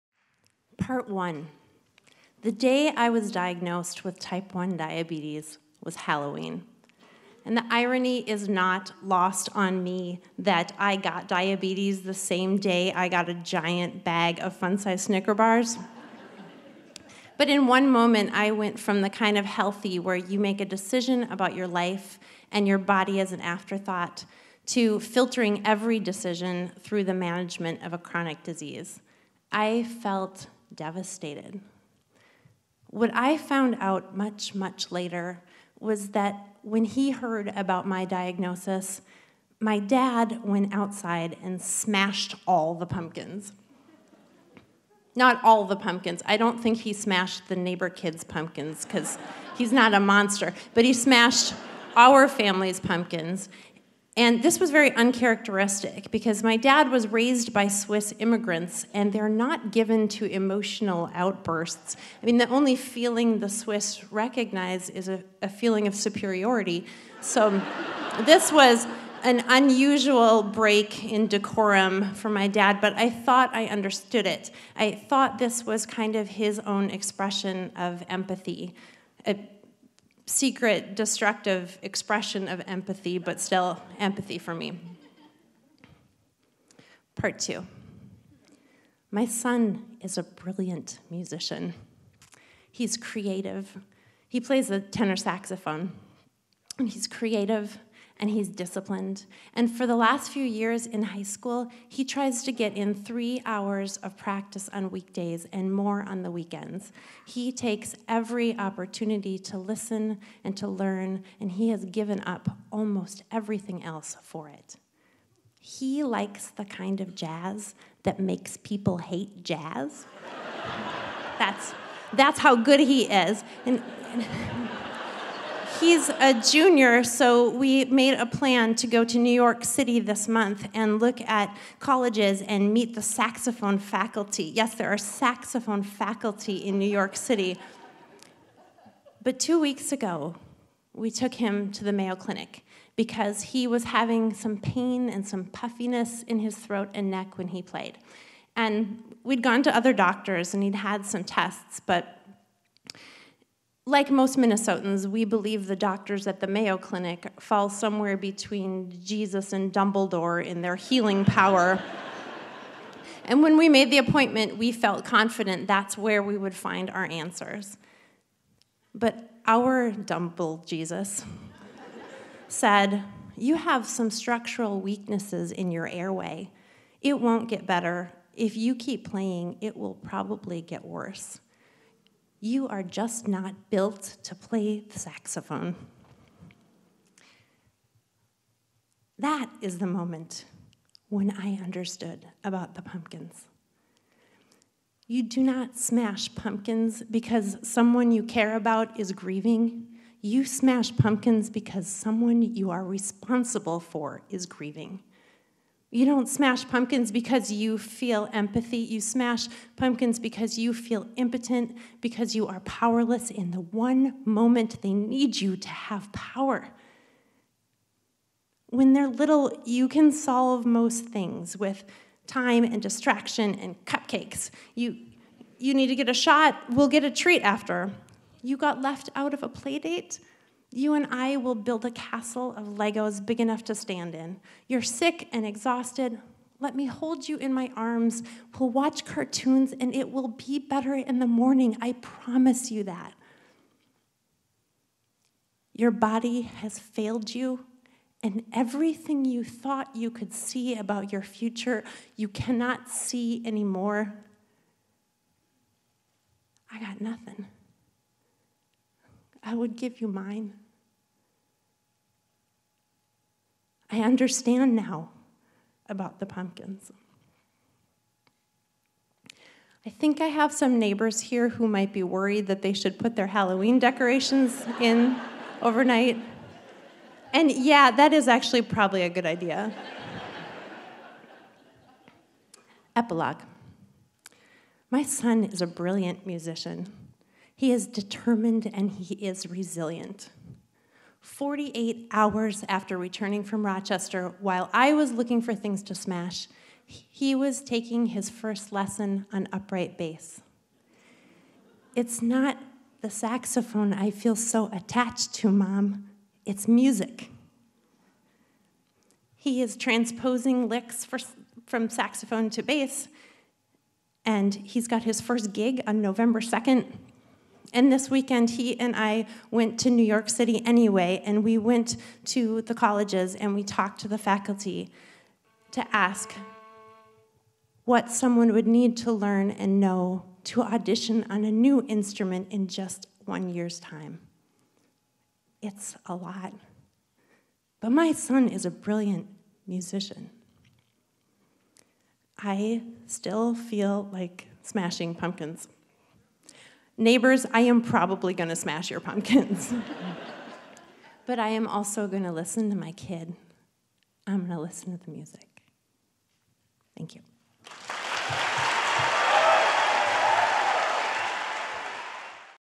Moth GrandSlam: Smashing Pumpkins
Official, better quality video of my GrandSlam story about smashing pumpkins, saxophones, and terribleness.